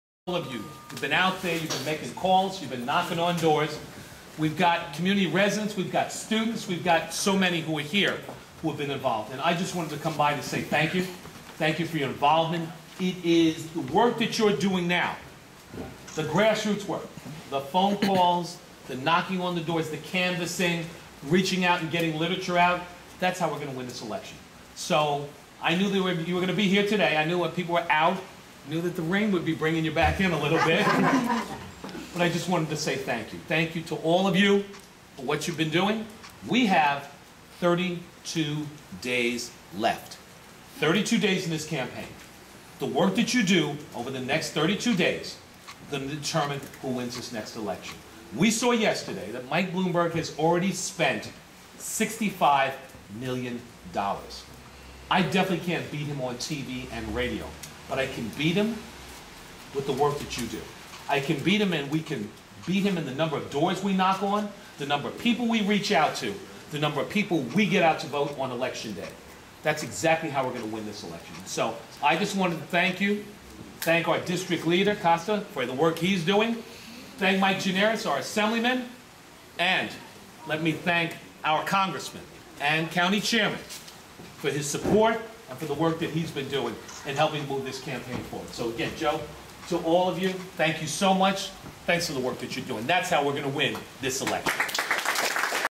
Bill Thompson in Astoria
Speaking at a Democratic club in Astoria on October 3, 2009.